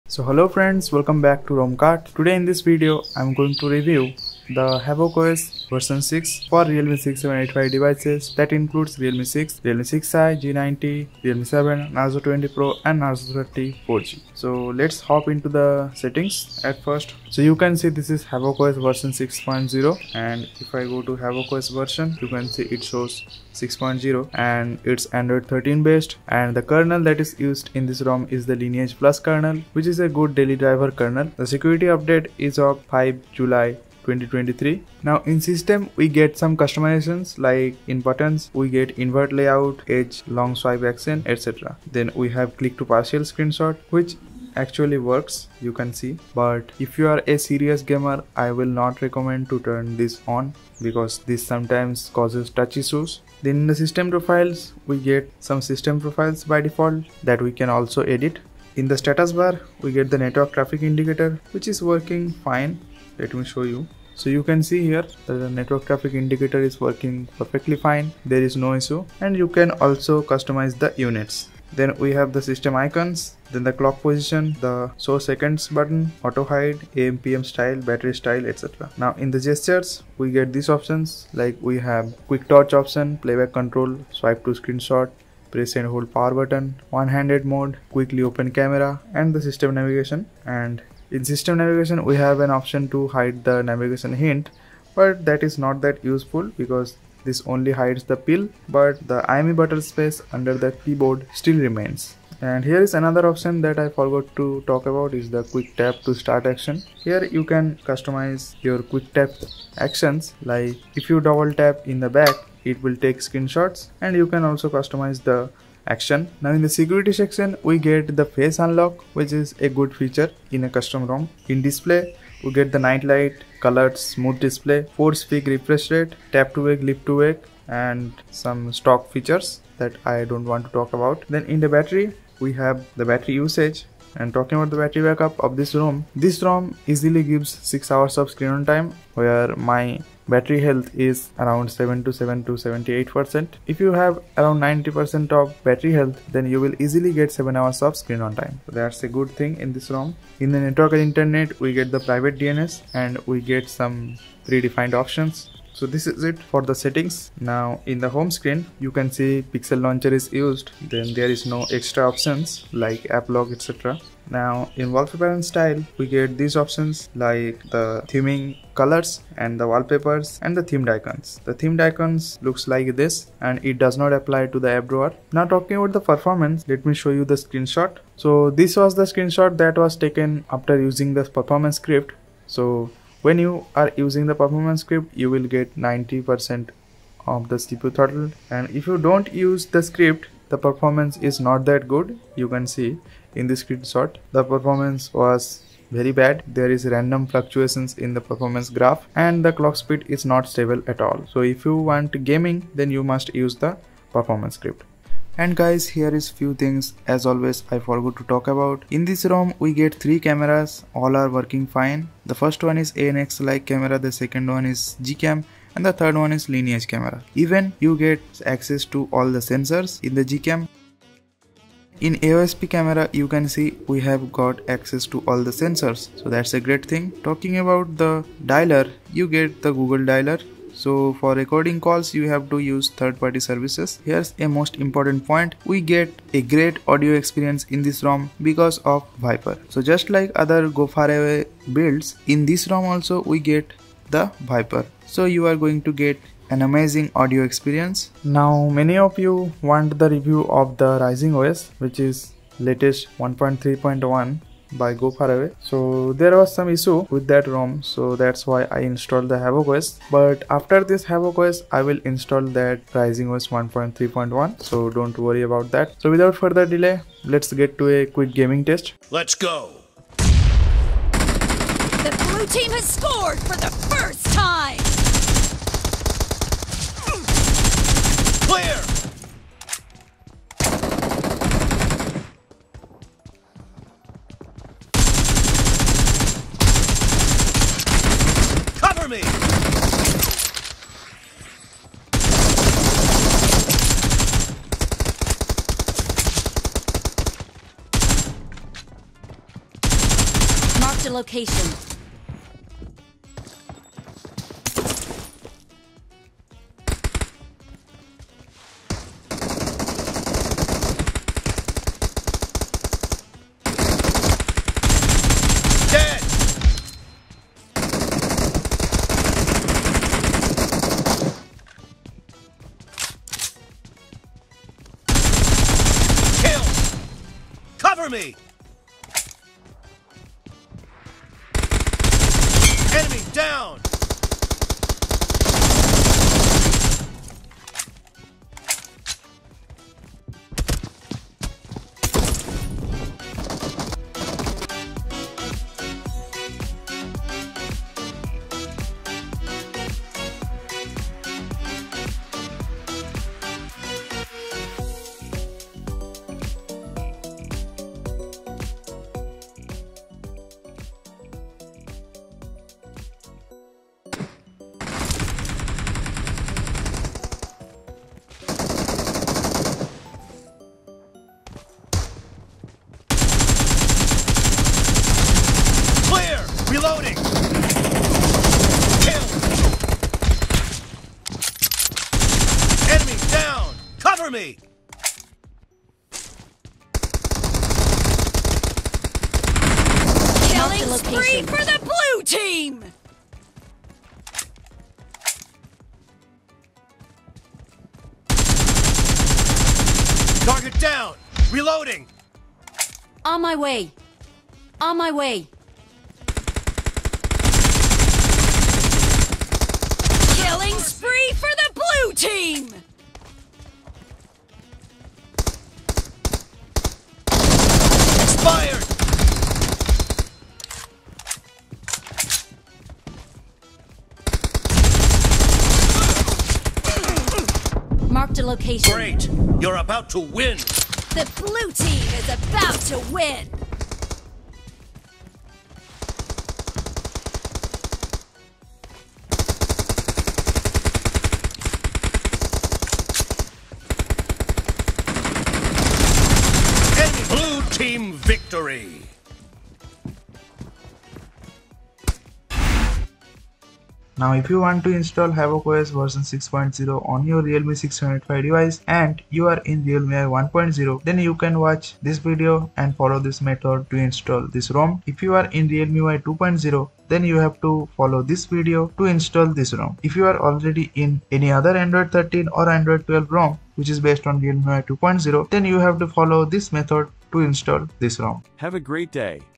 Havoc OS 6.0 for Realme RM6785 devices
Hey guys what's up today in this video I've reviewed the Havoc OS 6.0 for Realme RM6785 devices that includes Realme 6, Realme 6i-G90, Realme 7, Narzo 20 Pro, Narzo 30 4G.

❇️About & credits\_____________________________________________________
HavocOS 6.0 for Realme 6/6i(Indian)/6s/7/Narzo 20 Pro/Narzo 30 4G
• Build Type: GApp's
• Size: 1.9 GB
• Android Version: 13 (T) QPR3
• Security patch: 05 July
• Kernel : Lineage+ 320 with ksu support

Tested by Self Dev (Realme7)
By @Gofaraway71
Follow @rm6785_unofficial
Join @rm6785_gofaraway